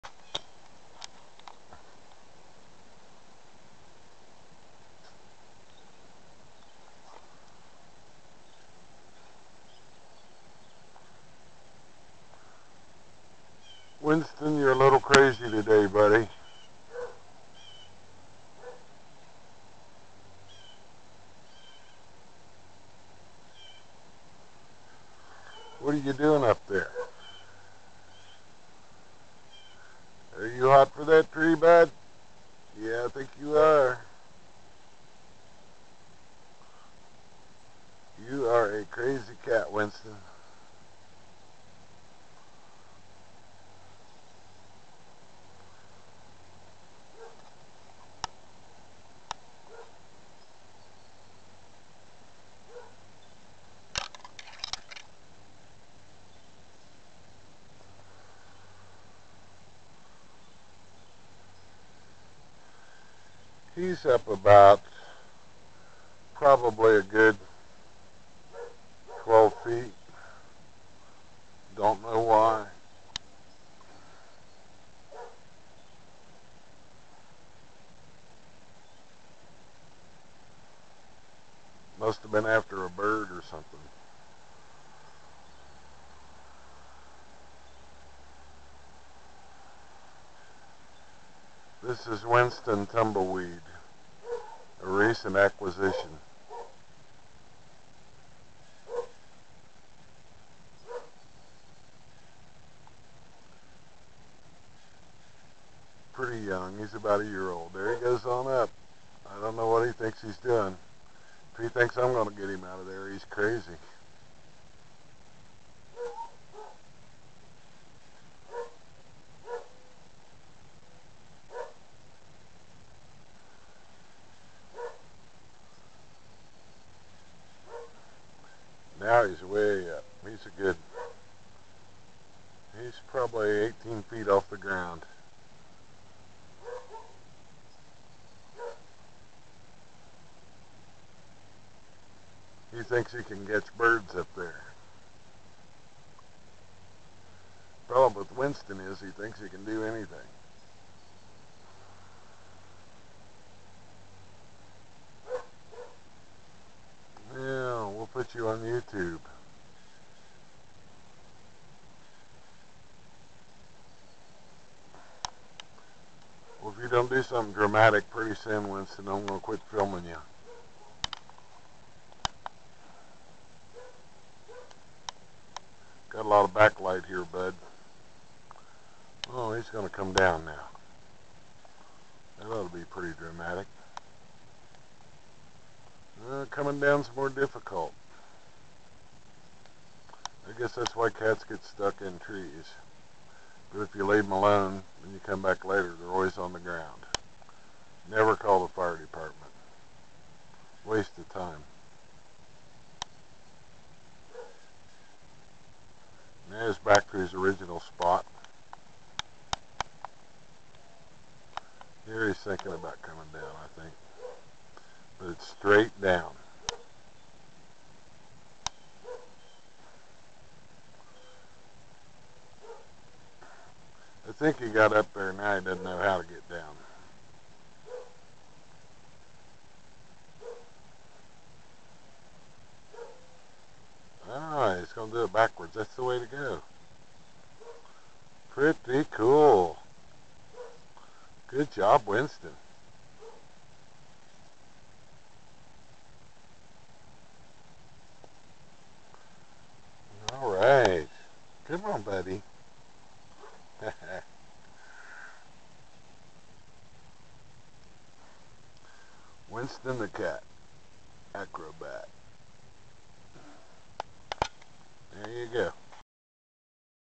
Never call the fire department...
Winston Tumbleweed is our newest orphan Tomcat -- he wandered in about a month ago, unchallenged by our two old long-resident outdoor Toms. Since that time he has made this weeping willow tree in the back yard his primary clawing post -- he has a real thing for it. Anyway, here you can see why you do *not* need to call for assistance to get a cat out of a tree. They can figure it out for themselves, given time.